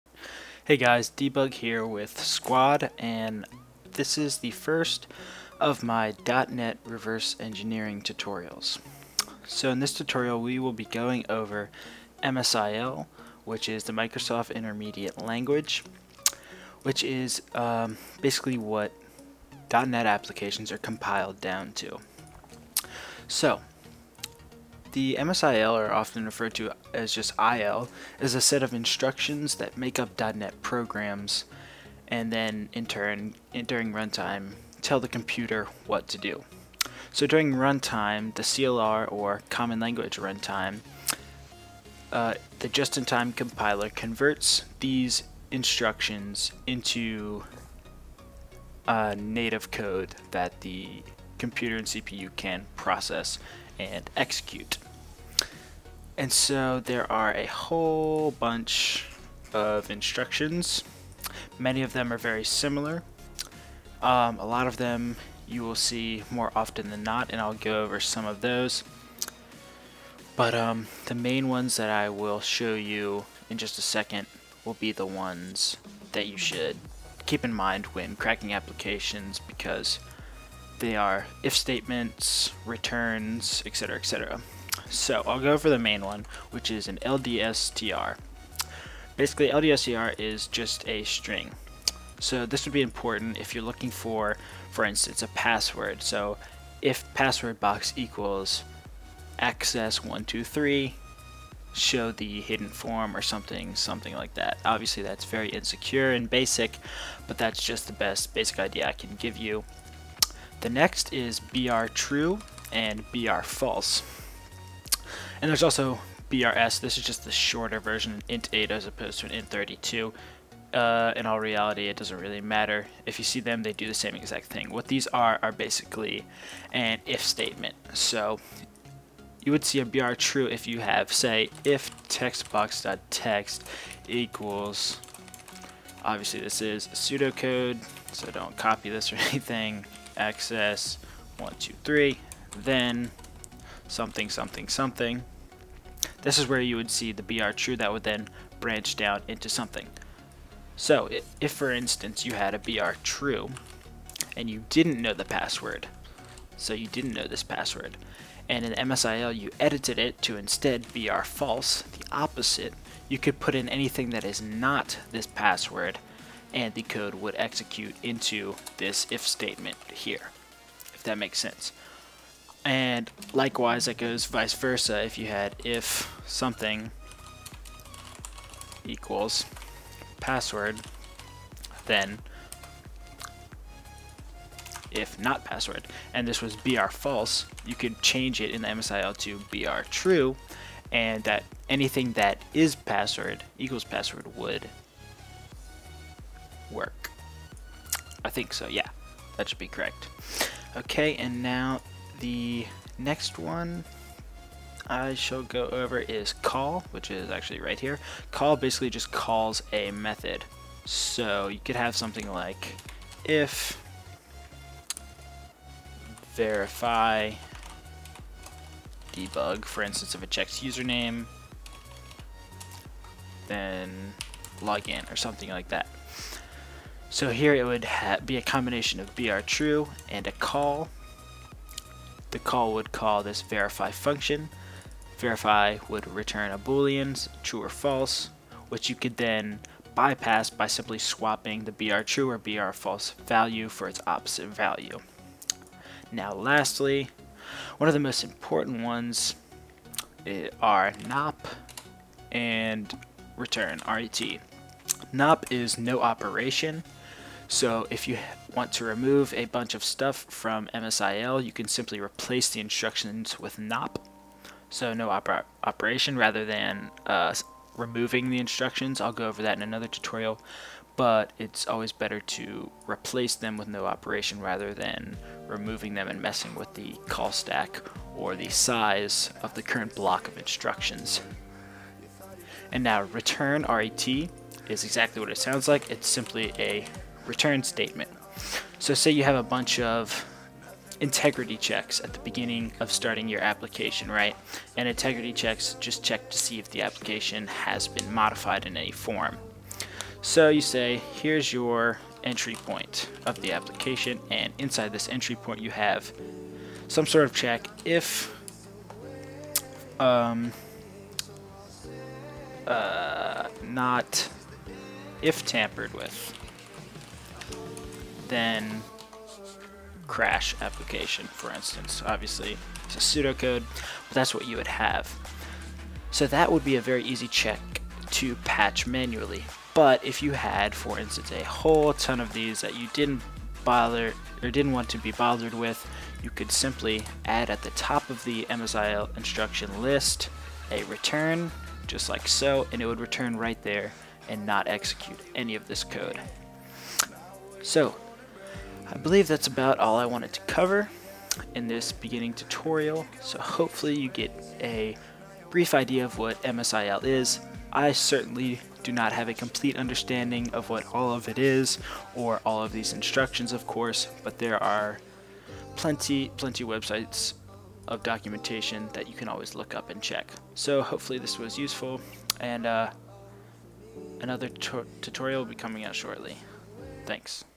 [Ep 1] .NET Reverse Engineering - The Basics of MSIL
This video outlines the very basics of MSIL to help you understand what you're working with when cracking .NET applications.

I certainly do not know everything about MSIL, as there is a lot to understand, and tons of instructions. One main thing I forgot is instructions have Opcode and operand. OpCode is the "ldstr" and the operand would be "string123".